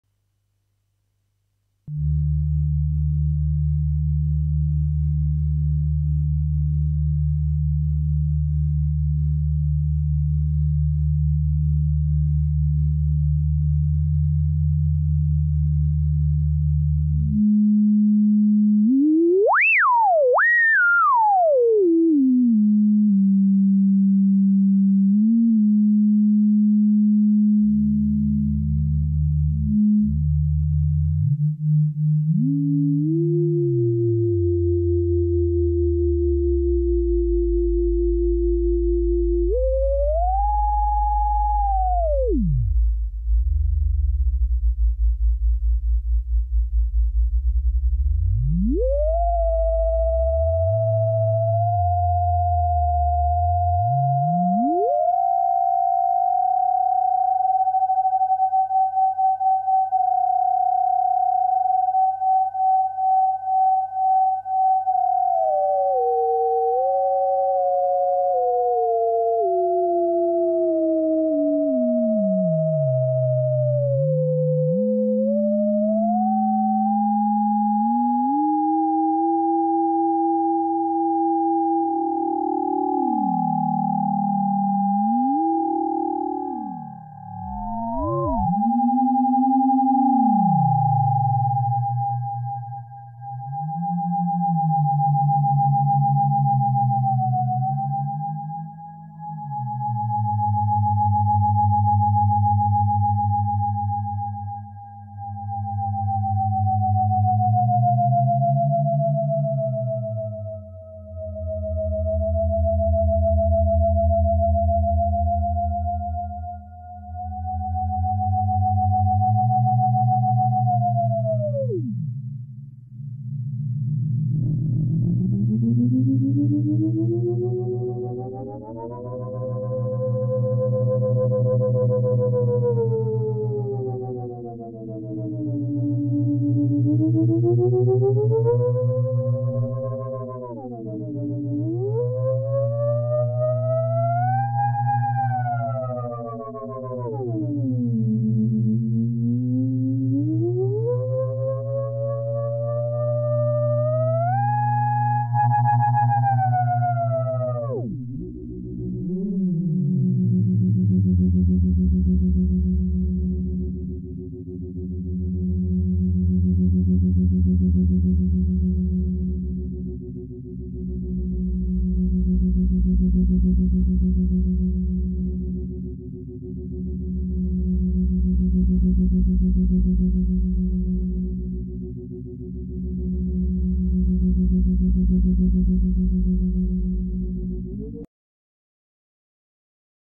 Pitch Shifting with Test Oscillator (Pure tone test)
I'll upload the aliasing from 20khz+ in sep video